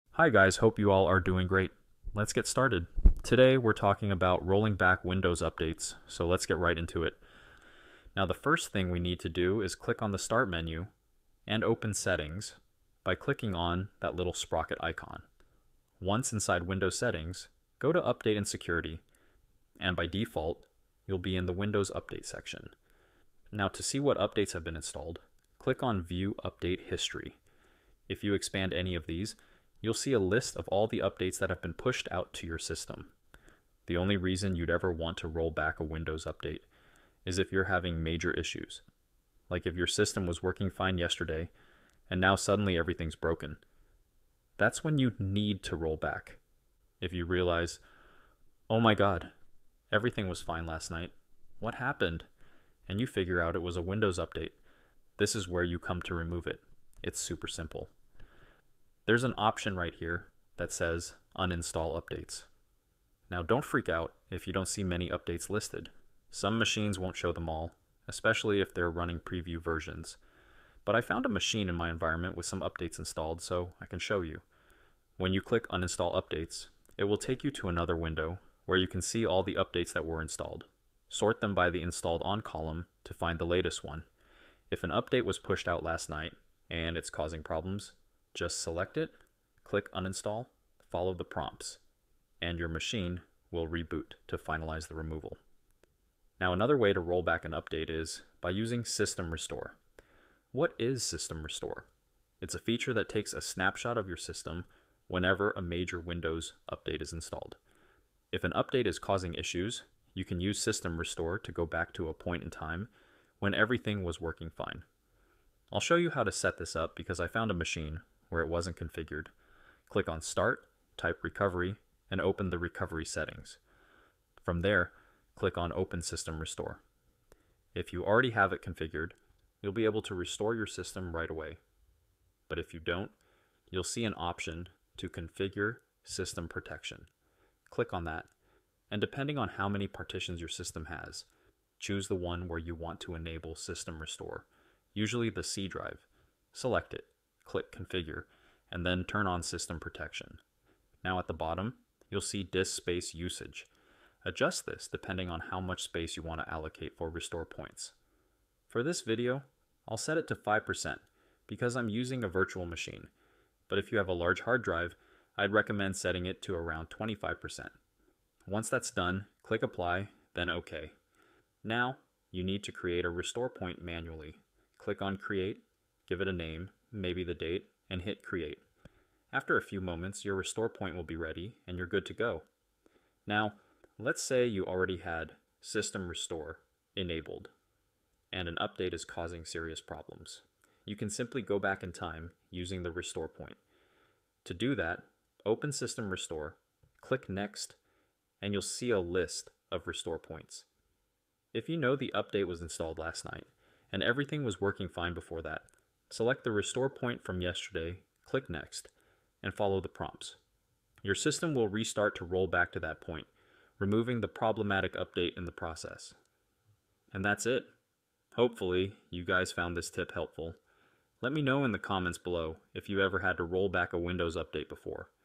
How to Go Back to a Previous Windows Update (2026)
NordVPN.

Having issues after a Windows update? In this step-by-step tutorial, we’ll show you how to roll back to a previous version of Windows to fix bugs, performance issues, or compatibility problems. Learn how to use recovery options, uninstall updates, and restore your system for a smoother experience.